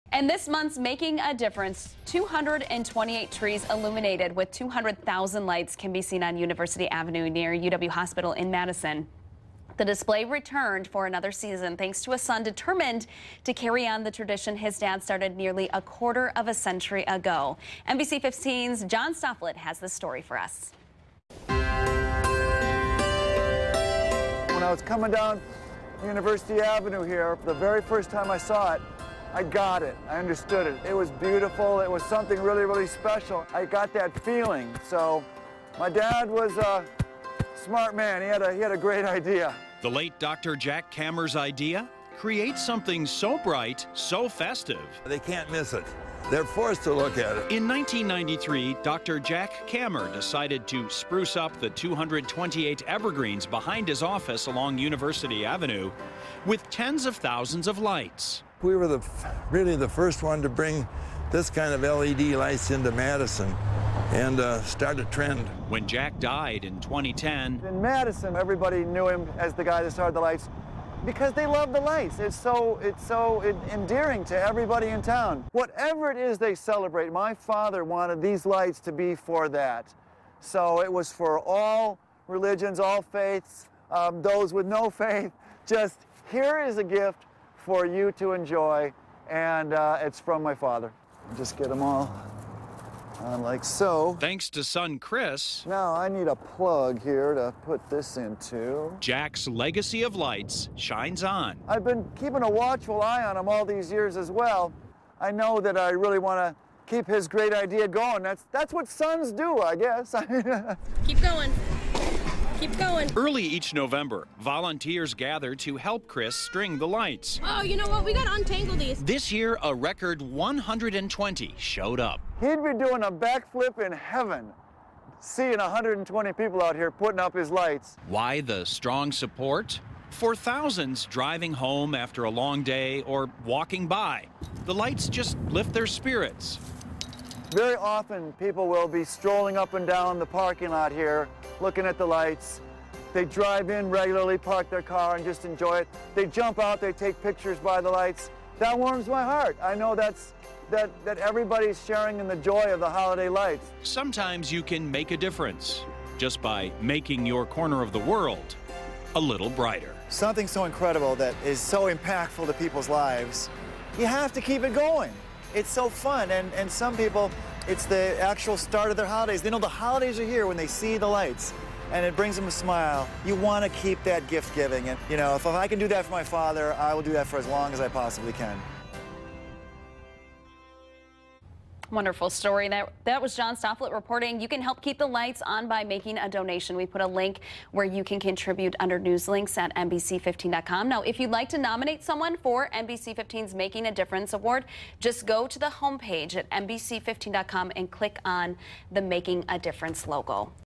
The Man Behind the Lights 11-25-15
John Stofflet tells us about the late Dr. Jack Kammer who began the tradition of the University Avenue Holiday Lights.